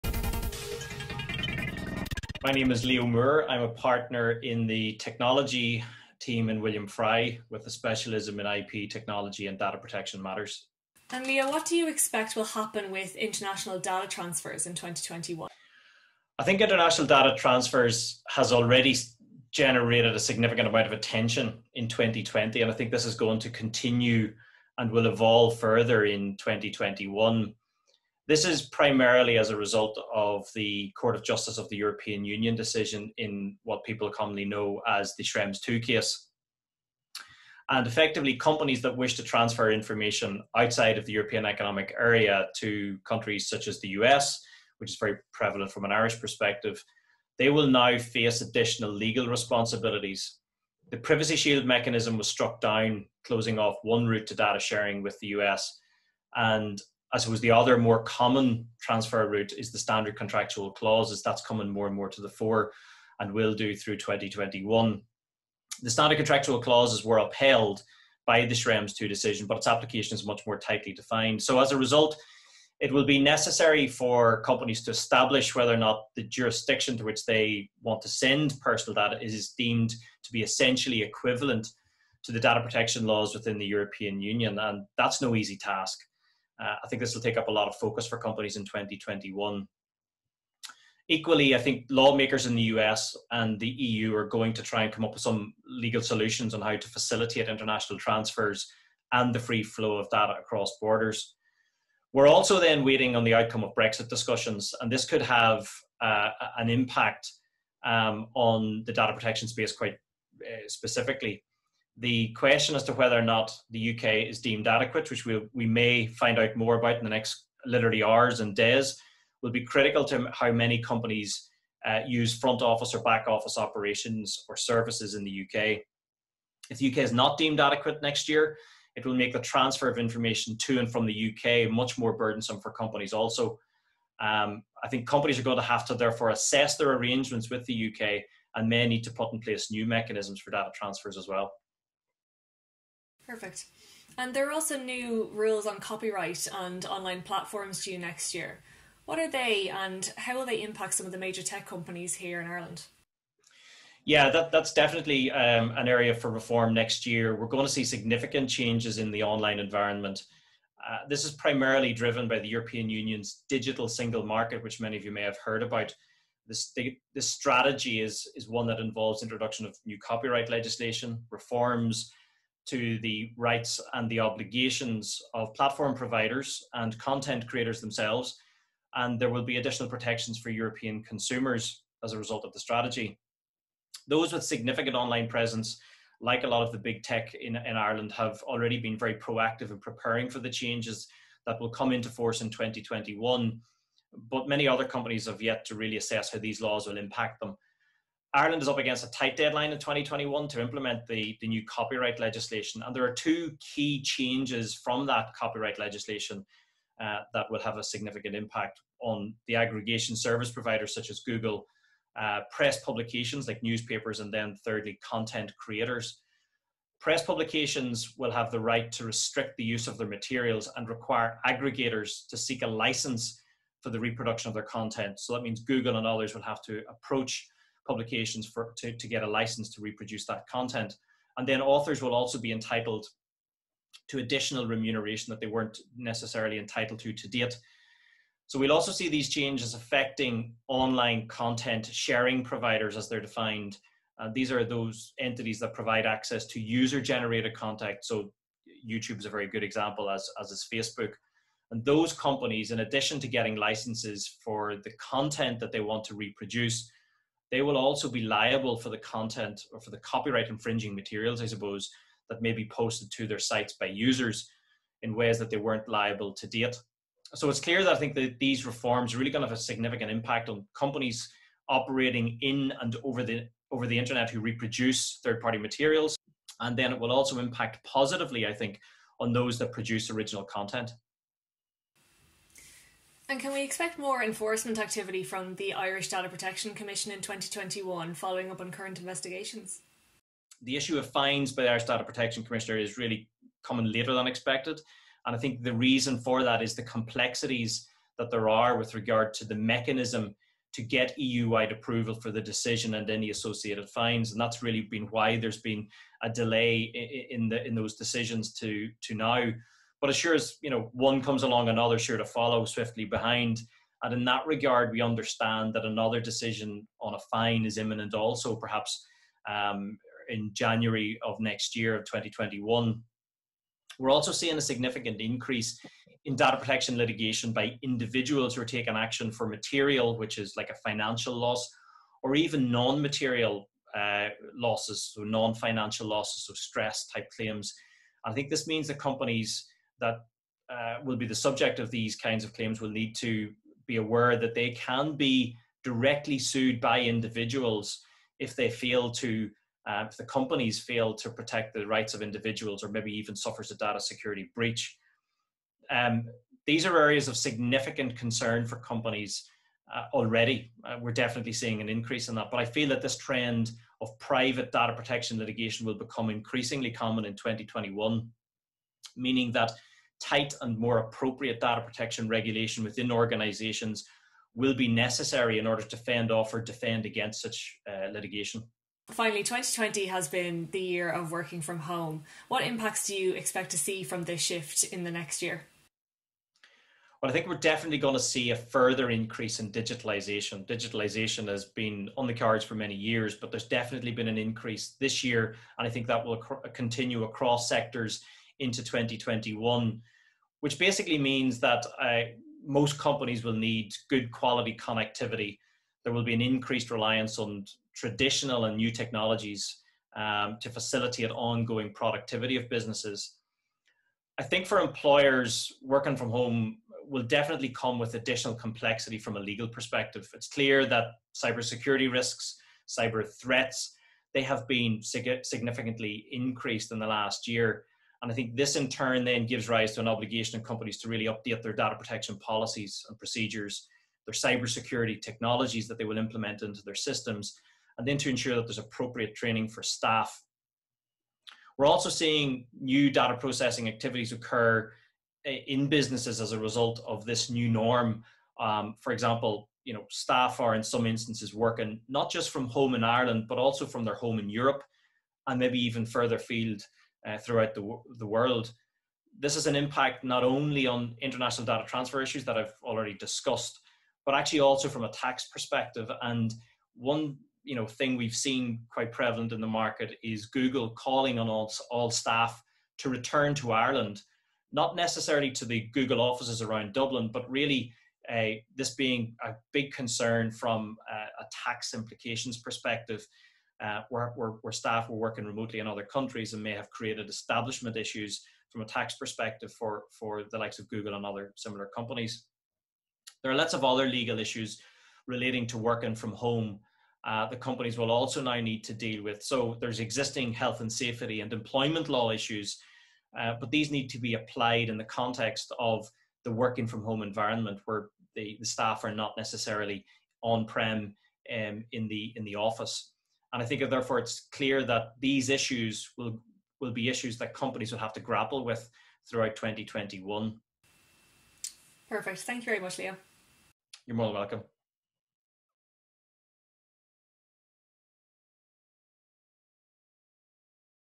Tech Trends 2021: Leo Moore, William Fry
Leo Moore from William Fry’s technology group looks at some of the legal and regulatory challenges facing tech in 2021, from international data transfers and new copyright rules, to issues arising from the spread of remote working.